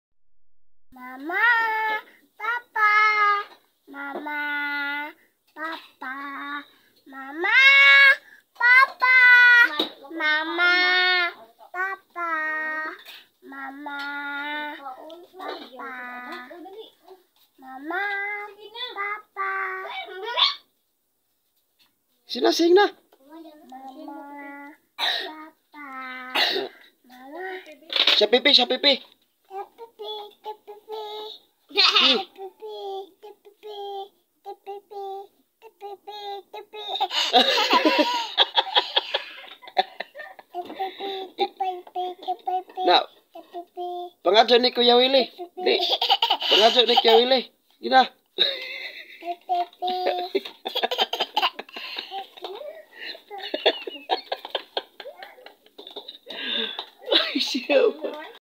Mama papa
may little girl sing mama papa  how great listening her voice very impressive